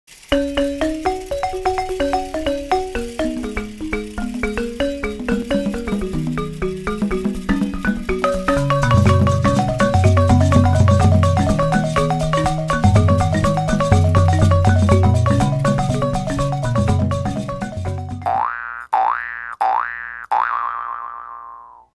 Pango Pango African Dancer
Vintage tin wind-up toy made in Japan by TPS (Tokyo Playthings)  1956

ToyTent.com